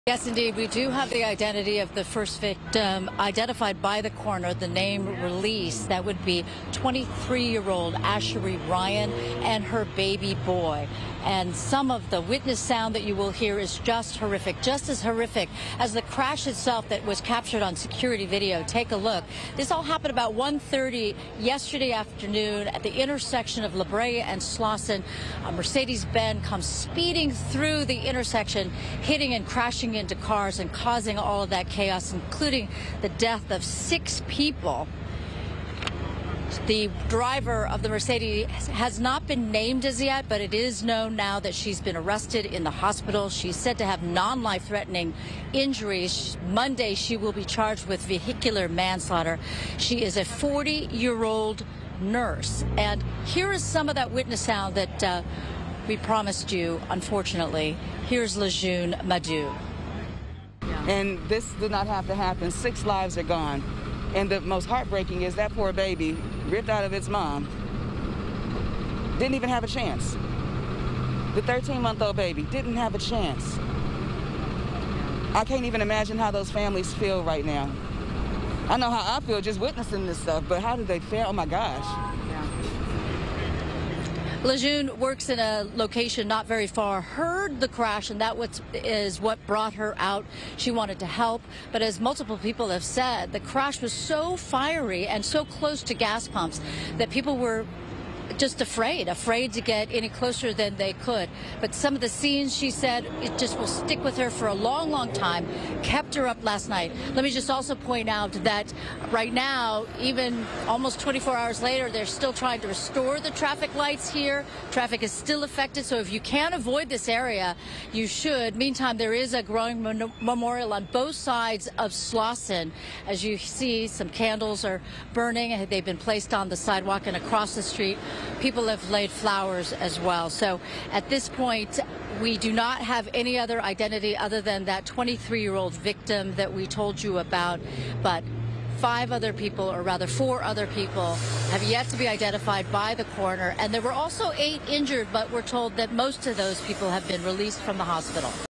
Windsor Hills crash: Pregnant woman killed in fiery collision identified
Asherey Ryan and her unborn child were among six victims killed in a fiery multi-vehicle crash caused by a speeding car that ran a red light in Windsor Hills. According to her family, her young son was also killed. STORY